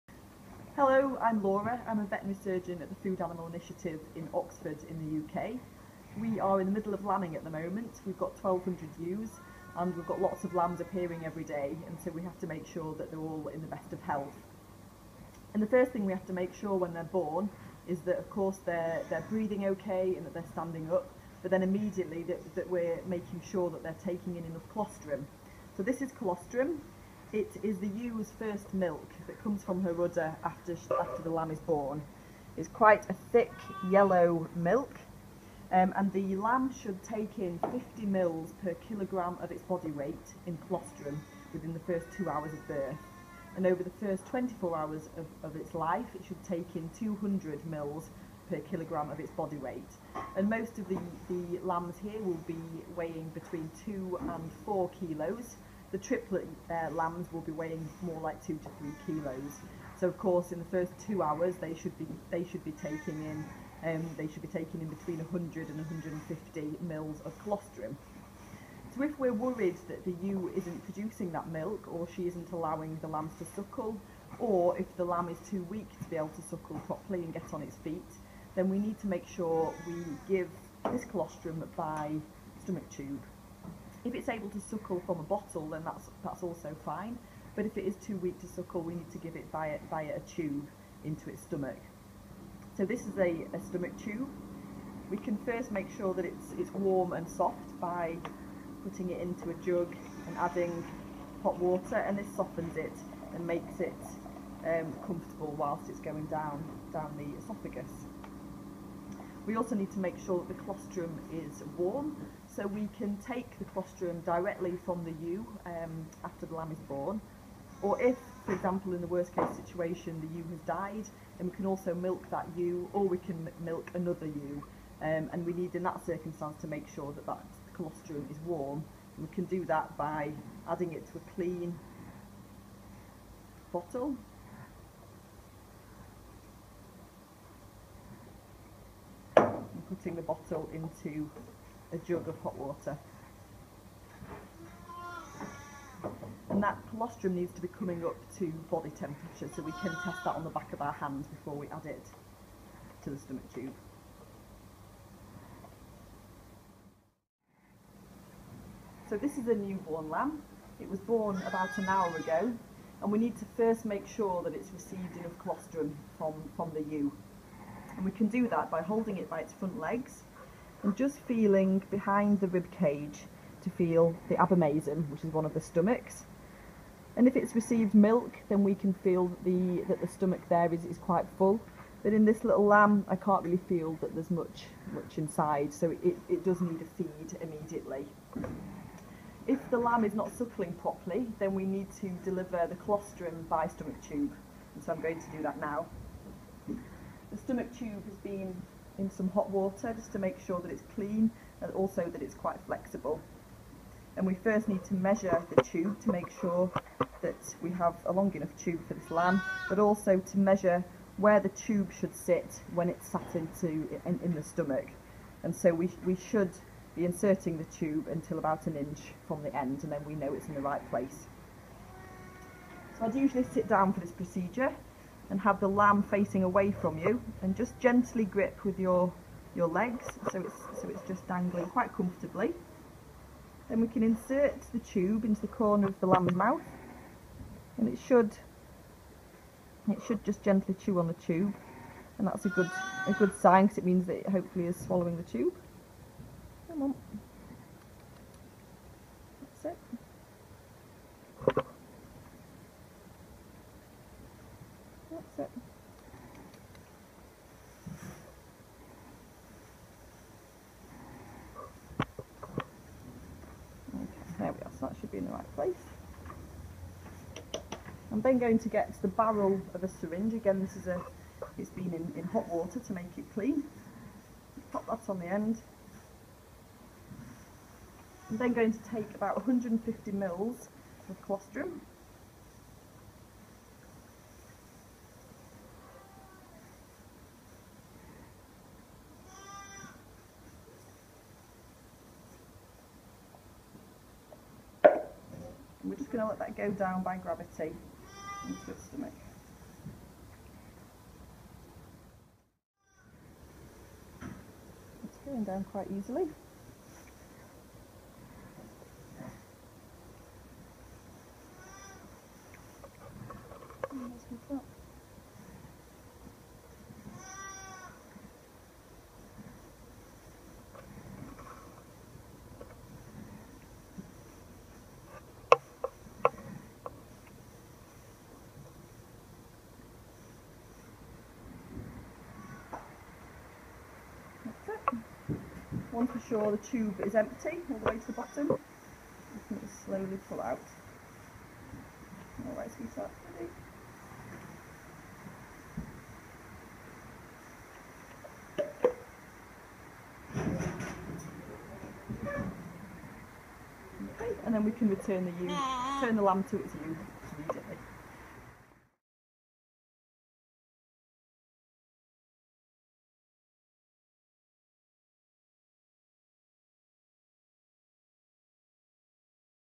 Tube-feeding a newborn lamb - FAI
Laura Higham, FAI Farms veterinary surgeon, explains how to tube feed a newborn lamb.